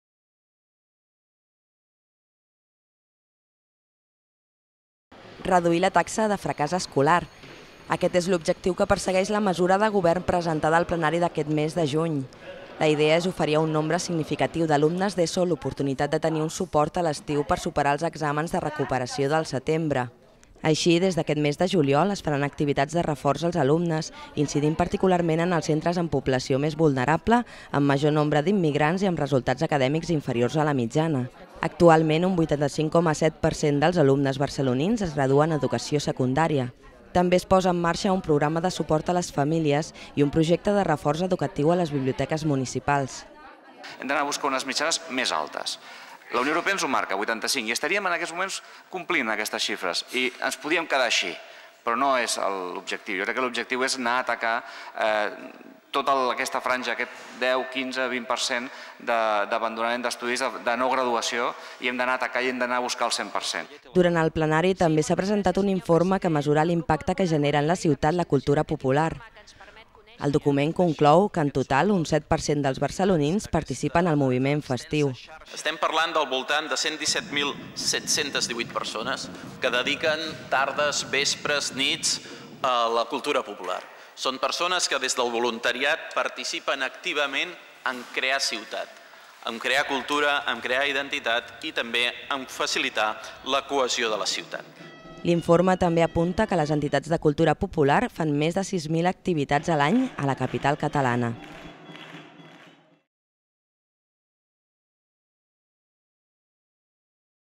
Barcelona oferirà hores de reforç a l'estiu als alumnes d'ESO amb assignatures pendents
La mesura, presentada al plenari d'aquest mes de juny, persegueix augmentar la taxa de graduats en secundària, tot i que actualment ja acompleix les exigències de la UE.